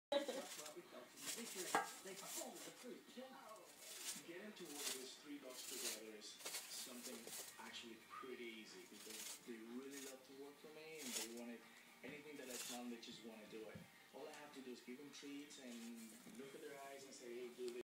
Chinchilla sand bath
Dexter takes a sand bath